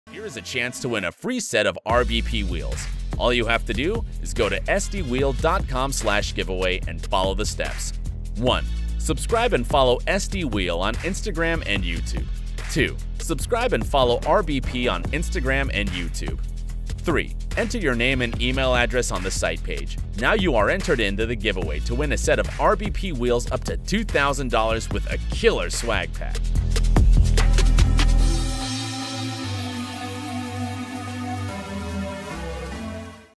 Win A Set Of Free RBP Wheels! (GIVEAWAY INFO)
Here's a quick look of a 20x10 RBP "Blade" wheel that you could get for FREE by entering our giveaway!